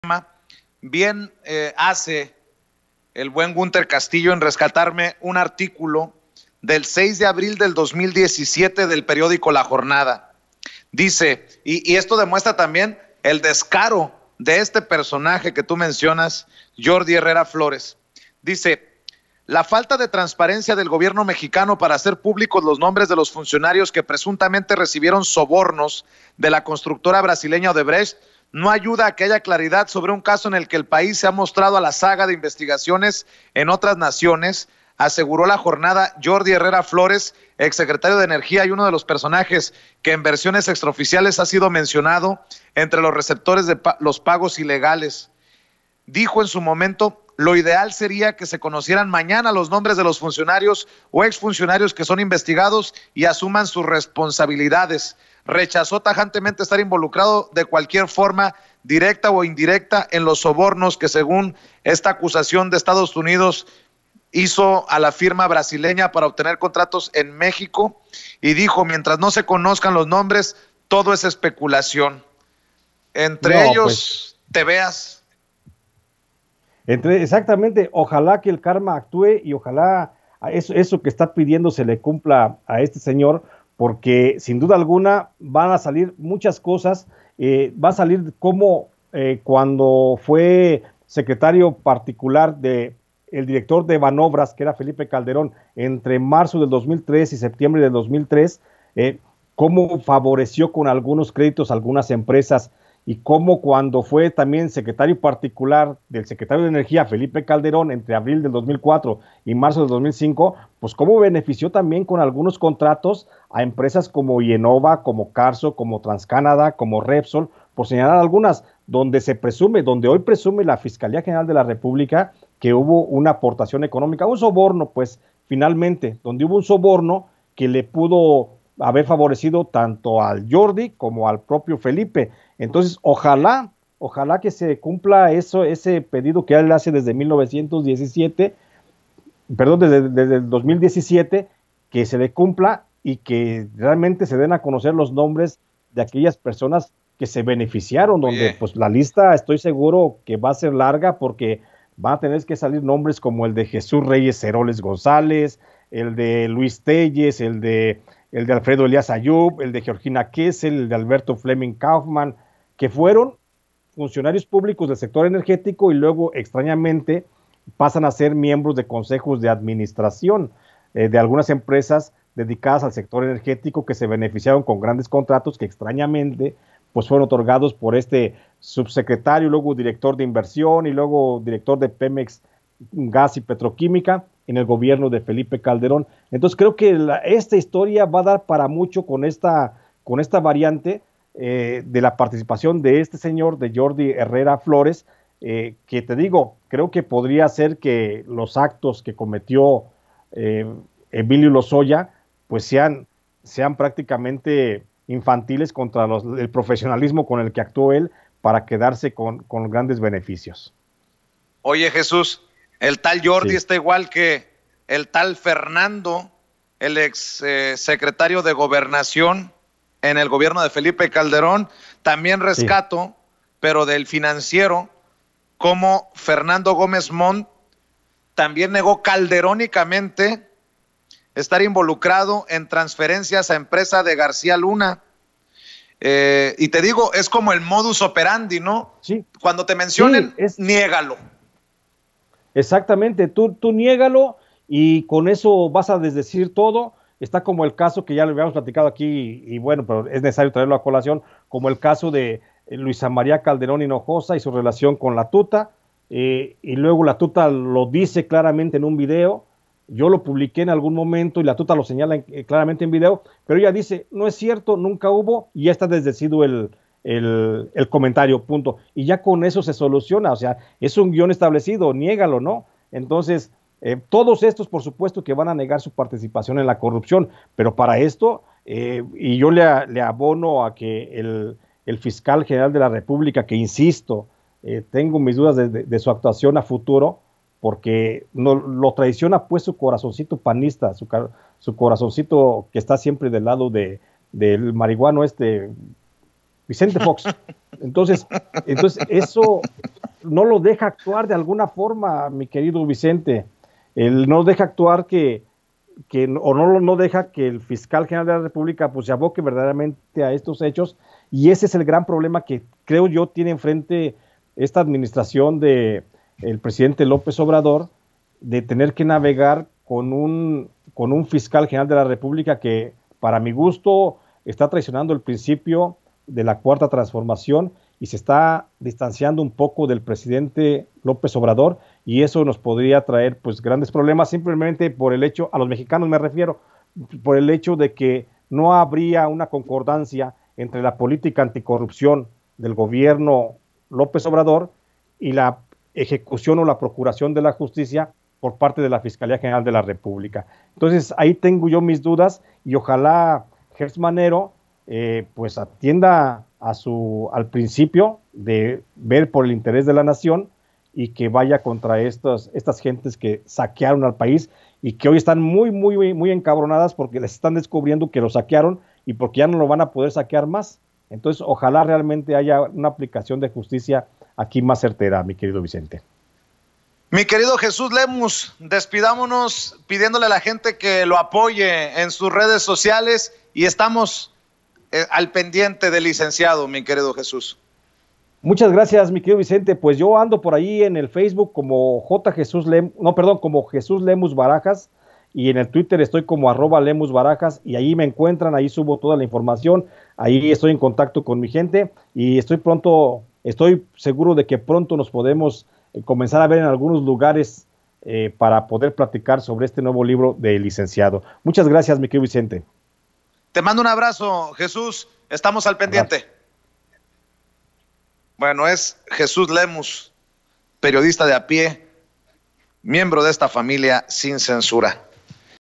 JESÚSLEMUS: CUANDO JORDY HERRERA FUE SECRETARIO DE CALDERÓN EN BANOBRAS FAVORECIÓ A EMPRESAS
JesúsLemus periodista nos dio a conocer que van a salir muchas cosas sobre Jordi Hernán Herrera Flores, puesto que cuando fue el secretario particular de Calderón en Banobras favoreció con créditos a unas empresas